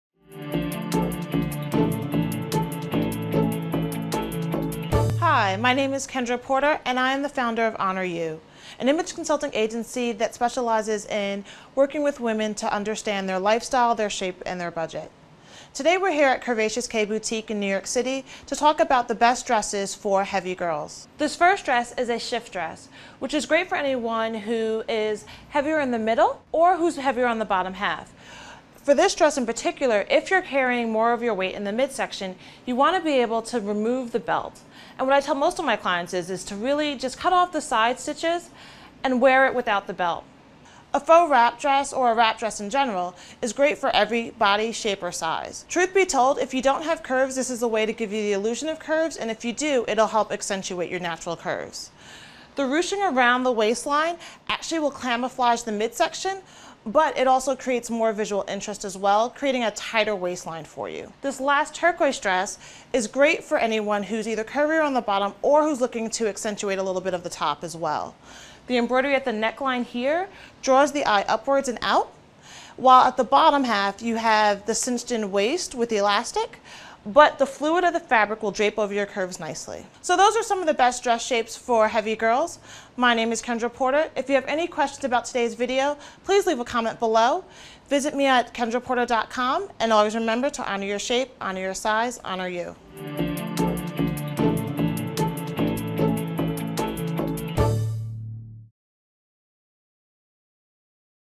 What Dress Shapes Look Best on Heavy Girls?
What Dress Shapes Look Best on Heavy Girls?. Part of the series: Fashion for All Shapes & Sizes. You always need to take your own physical characteristics into consideration when picking out a dress shape to war. Find out what dress shapes look best on heavy girls with help from a body expert and style architect in this free video clip.